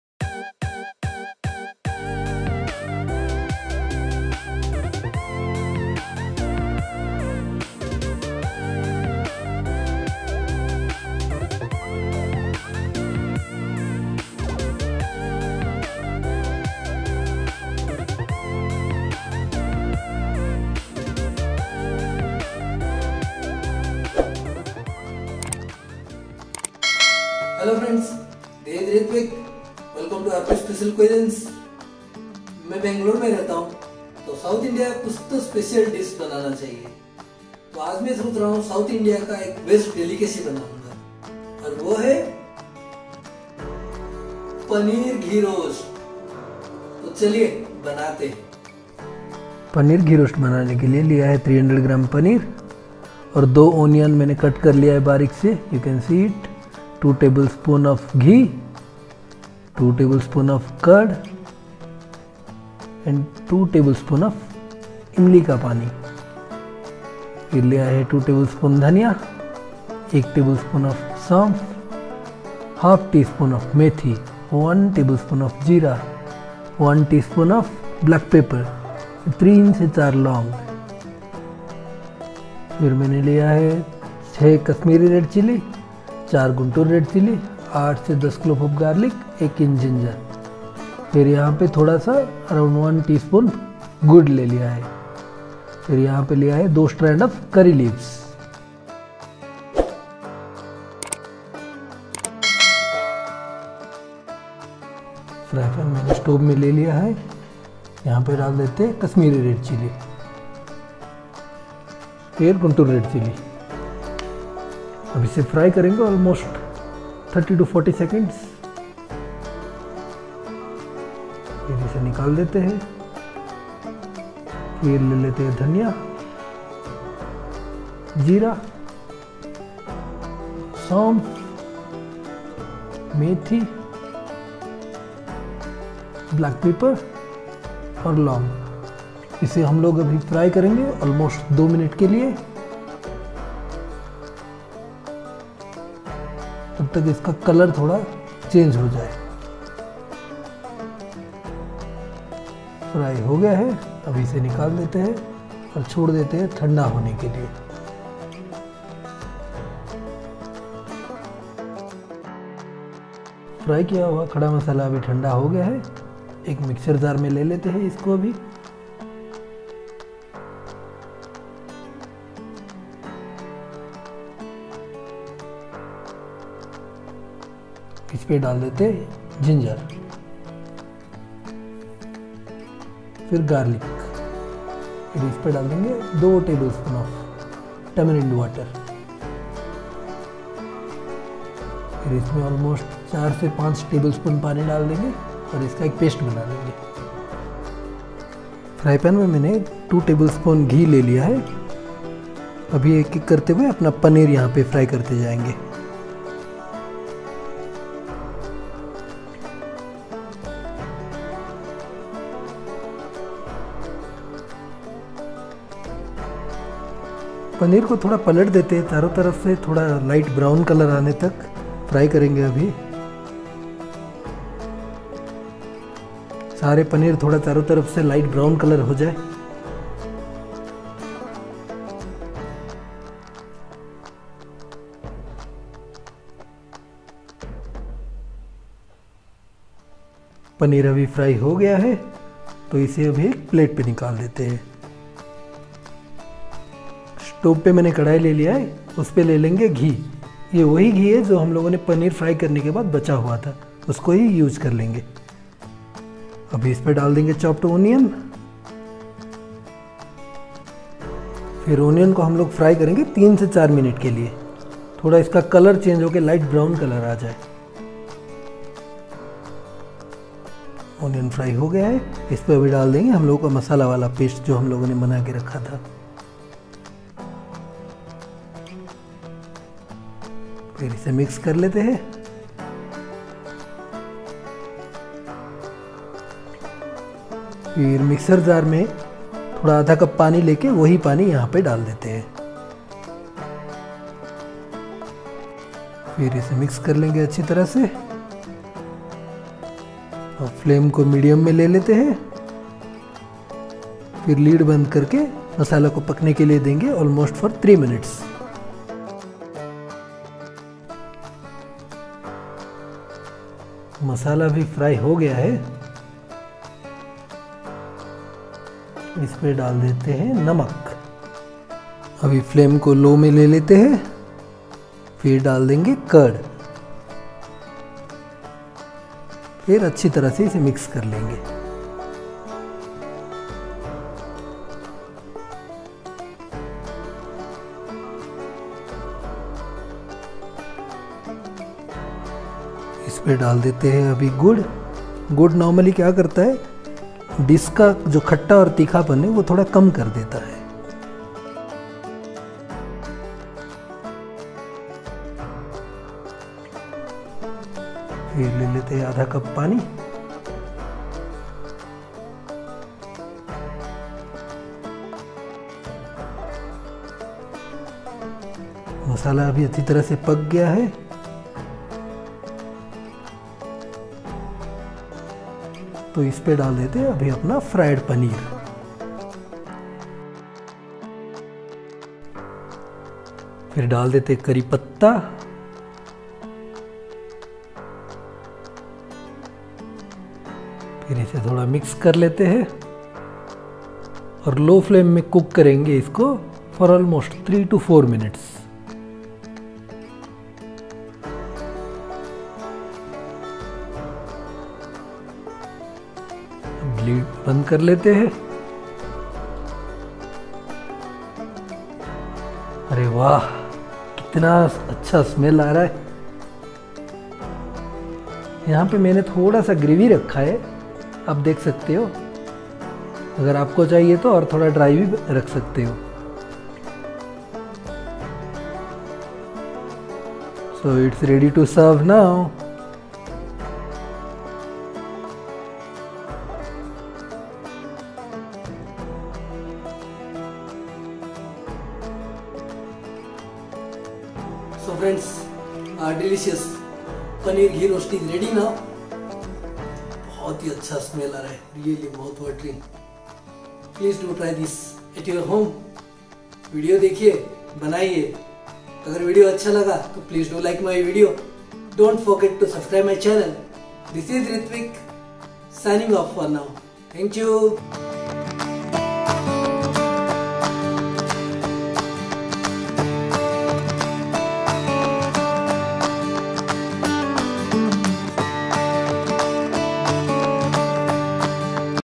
Paneer Ghee Roast Recipe | पनीर घी रोस्ट | Paneer Ghee Roast by RP's Special Cuisines
Paneer Ghee Roast Recipe | पनीर घी रोस्ट |  Paneer Ghee Roast by RP's Special Cuisines | How to Make Paneer Ghee Roast | Paneer Recipe

Preparation Time: 15 minutes.
Cooking Time: 20 Minutes
Serves:  4
Course: Maincourse
Cuisine: Indian

Ingredients:
• Paneer 400 gm (cubes)
• Onion 2 Big size (chopped)
• Ghee 2 tbsp
• Curd 2 tbsp (whisked)
• Sugar / jaggery 1 tsp
• Curry leaves 2 strands.
• Guntur dry red chilli 4 nos.
• Kashmiri dry red chilli 6 nos.
• Black pepper corn 1 tsp
• Fennel seeds 1 tbsp
• Methi seeds 1 tsp
• Cloves 3-4 nos.
* TSP: Tea Spoon. TBSP: Table Spoon.

Method:
• Dry roast the whole red chillies for 2-3 minutes, dry roast the other whole spices until their aromas are released.
• Add the roasted whole spices along with roasted whole red chillies in a blending jar, add garlic, ginger and tamarind water, grind into a fine paste, adjust the consistency by adding little water.
• Heat ghee in a pan, shallow fry the paneer from all sides until golden brown.
• Heat the ghee in a wok, use the same ghee used for frying paneer, fry the chopped onions until they are translucent.
• Add the freshly prepared masala paste, stir and cook on low flame for 1-2 minutes, add curd and keep stirring continuously.
• Increase the flame and cook until ghee is released. Add salt, sugar / jaggery to balance the taste, you can skip if u do not prefer sweet.
• Cook of another 2-3 minutes, add the fried paneer cubes, curry leaves. Coat the paneer with the masala and cook further 3-4 minutes.
• Serve hot.

Cookware set I have used for cooking & Photography